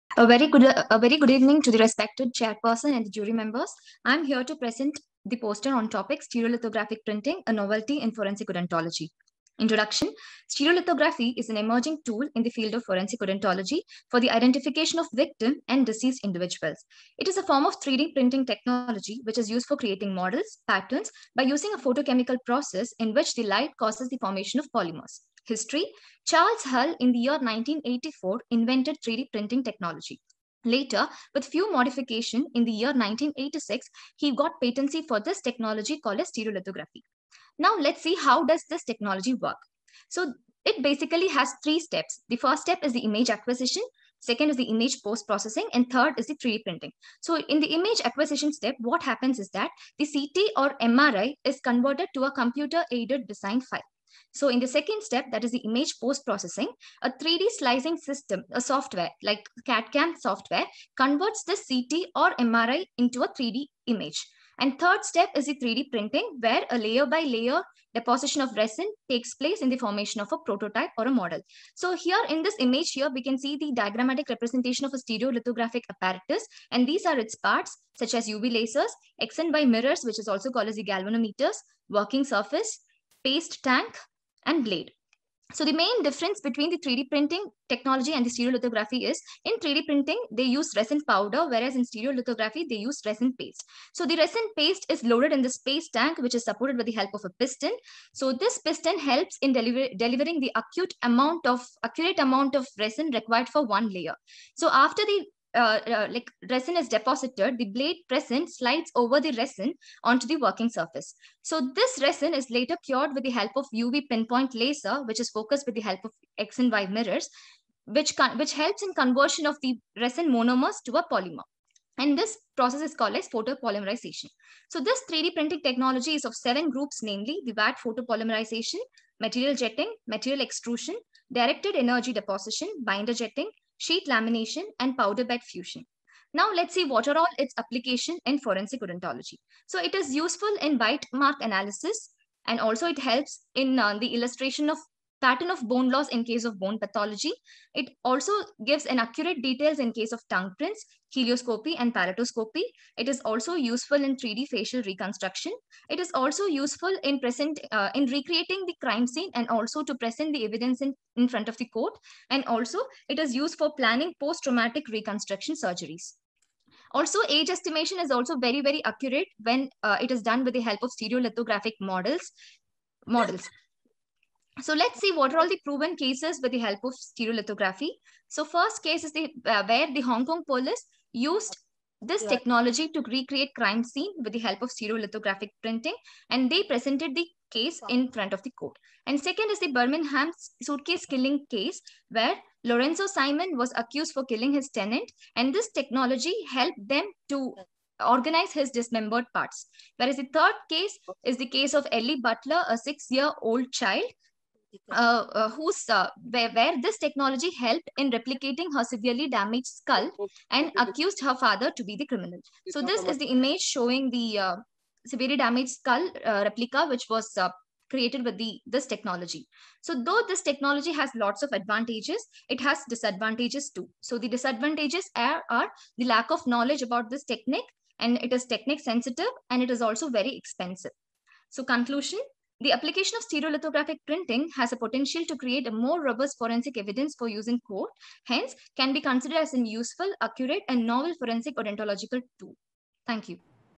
Steriolithographic Printing – A Novelity In Forensic Odontology: ePoster7
Forensic odontology is a challenging branch of forensic science that involves the application
of dental sciences in the identification of deceased individuals. Stereolithography is an
emerging tool in the field of forensic odontology for the identification of victims and the
deceased individuals. Stereolithography is a form of 3D printing technology which is used for
creating models, patterns, etc, by using a photochemical process in which the light causes the
formation of polymers by Priyanka Madhavan and Sudeendra Prabhu.